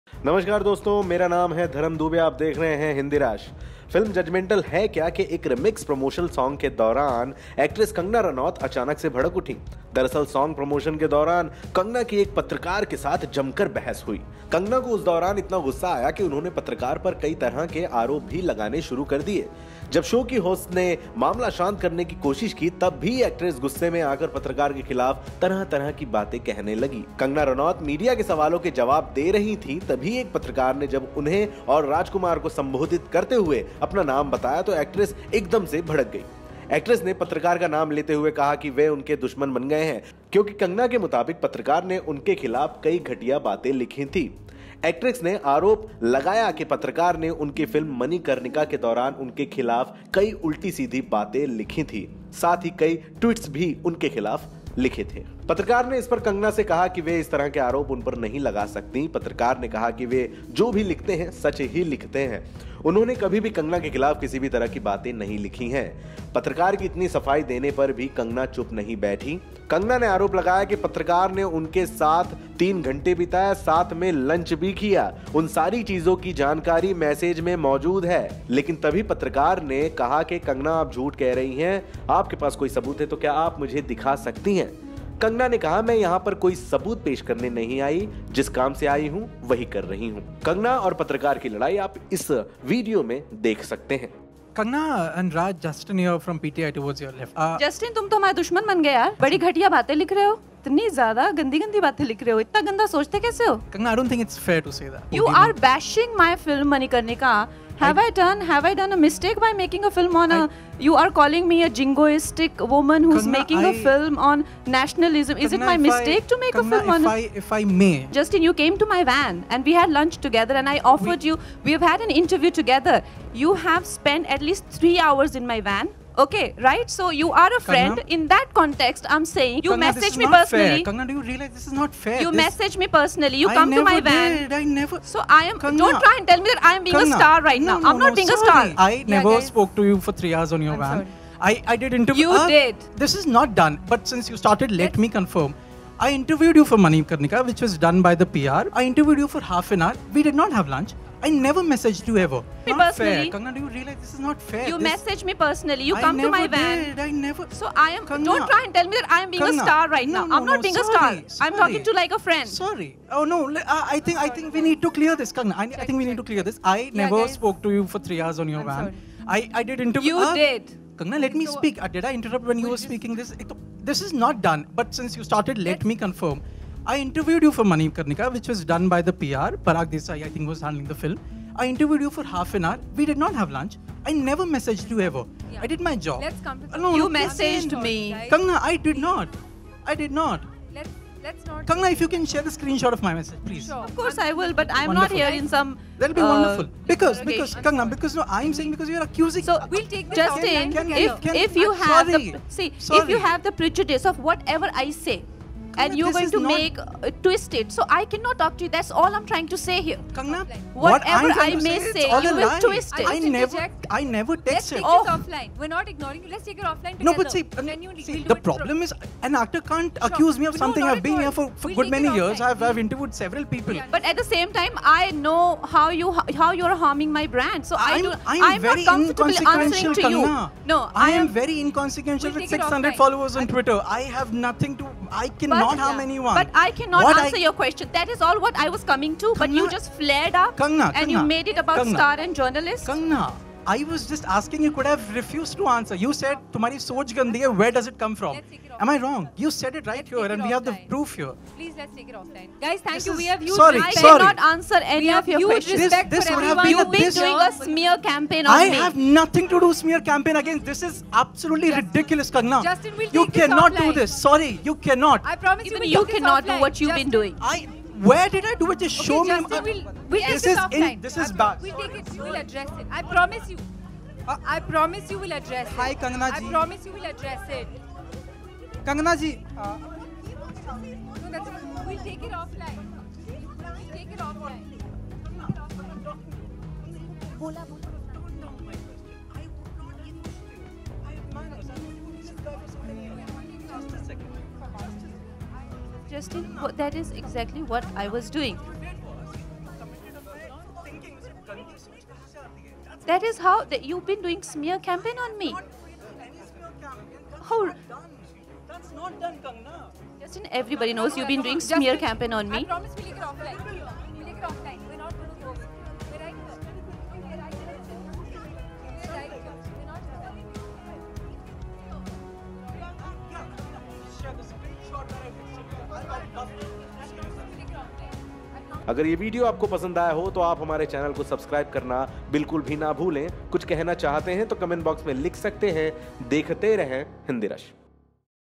Kangana Ranaut FIGHTS With Journalist, Why? Watch details | Judgementall Hai Kya | Rajkumar Rao
Welcome to HindiRush.com, Ekta Kapoor अपने प्रोजेक्स के प्रमोशन के लिए जिस तरह का फंडा अपनाती हैं वो तो हम सभी को पता है। Judgementall Hai Kya Movie Promotions के दौरान Kangana Ranaut का Reporter से Fight करना कहीं प्रमोशन फंडा तो नहीं? देखें Video